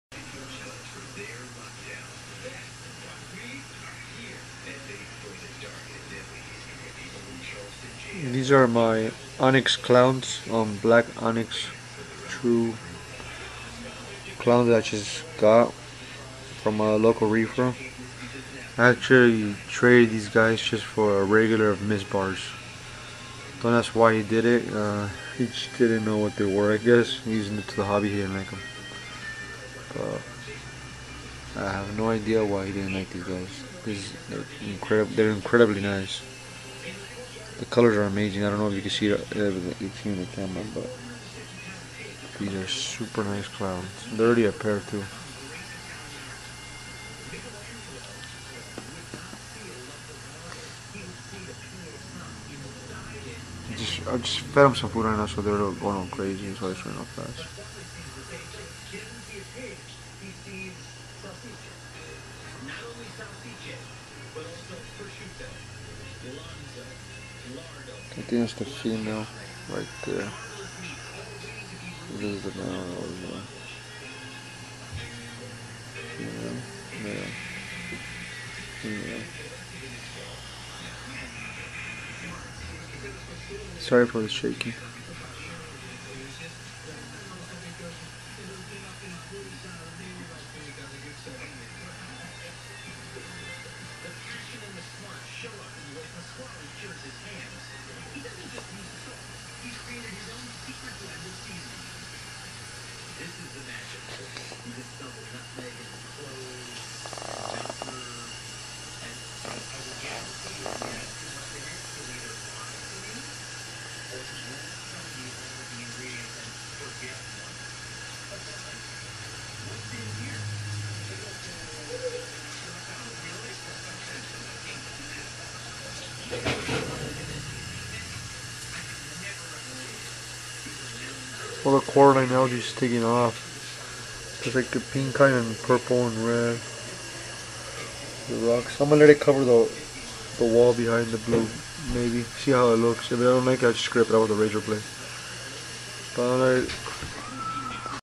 20 Gallon long reef tank
My new onyx clowns there is a female and male.